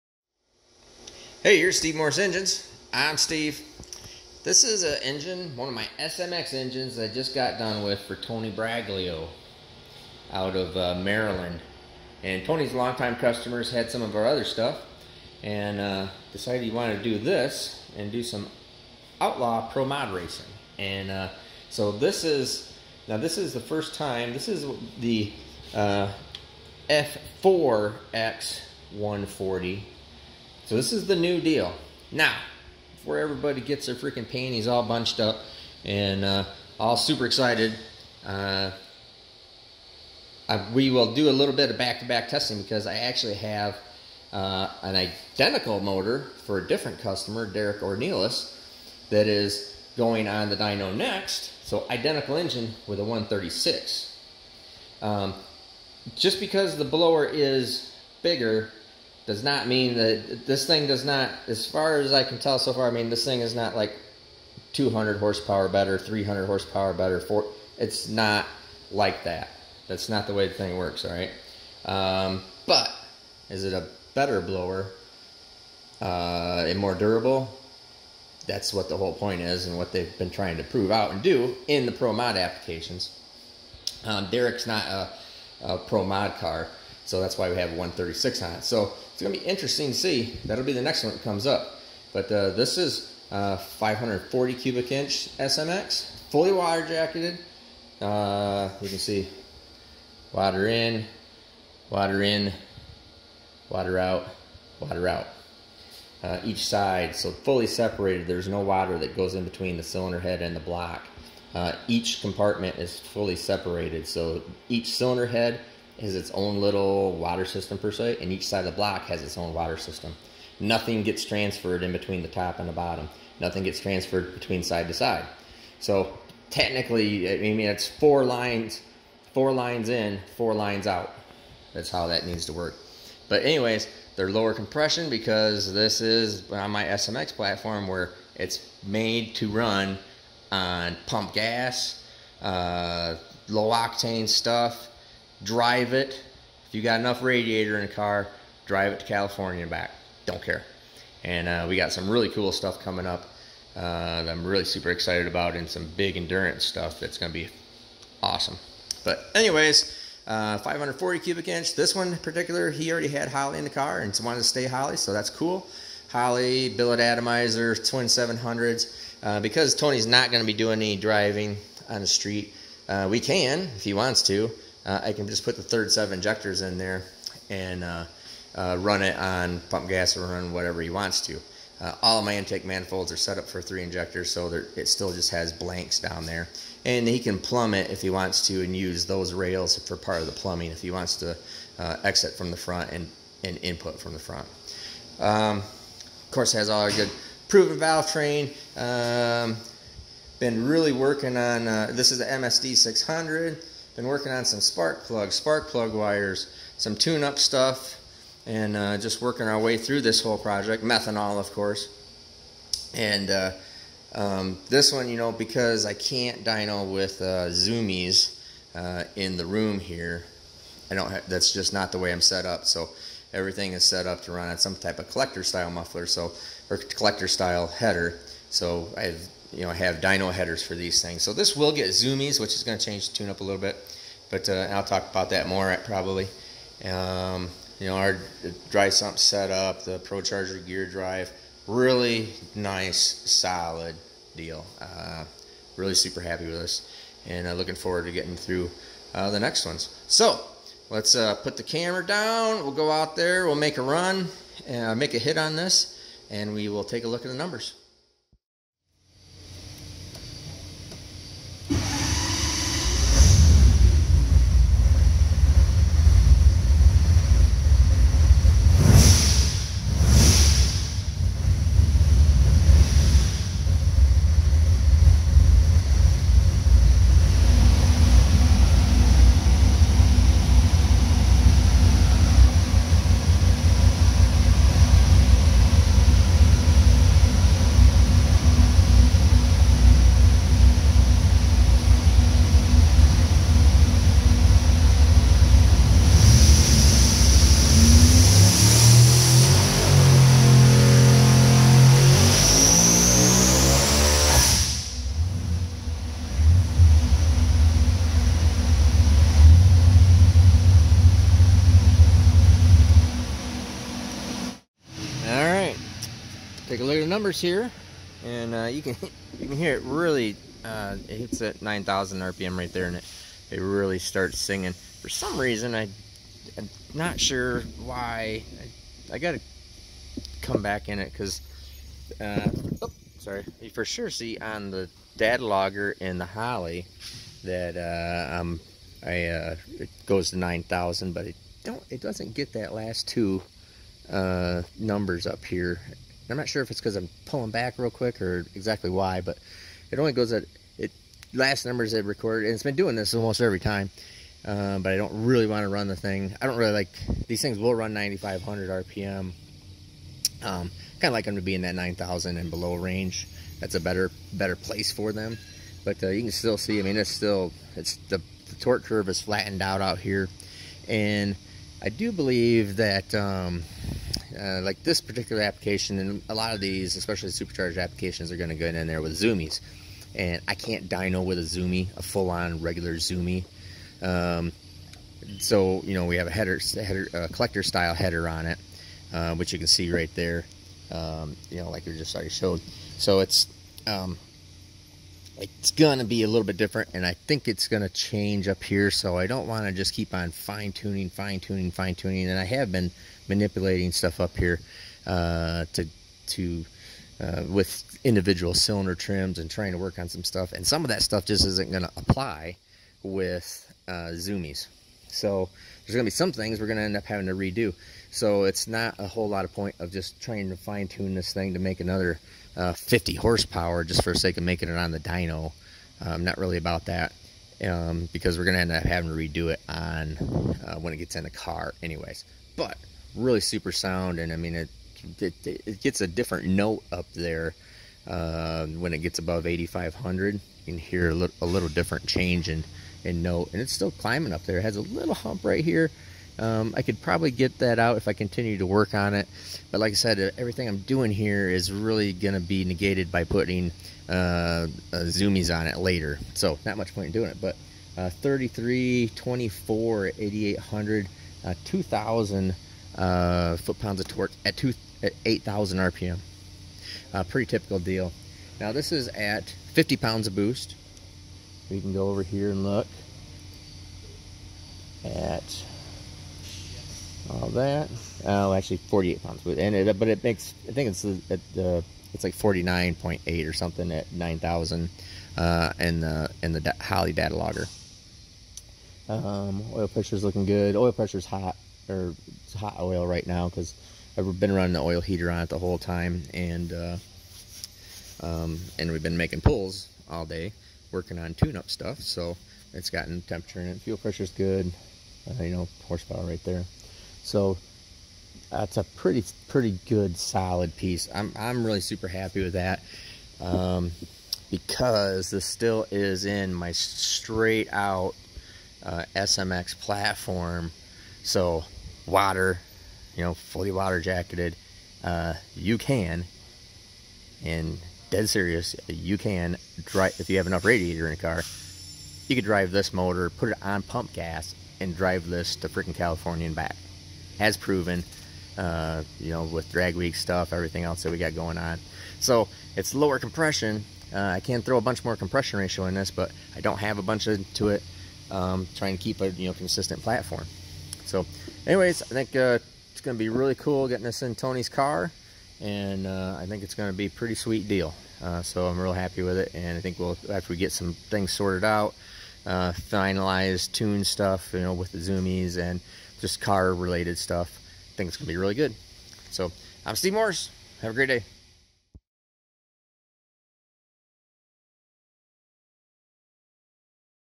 3,300+HP SMX with the NEW ProCharger F4X-140 Engine Dyno!!
This is another nice SMX engine that we just finished up for Tony Braglio.  We are using the new F4X-140 ProCharger and putting down over 3,300HP on Methanol at just under 50 pounds of boost.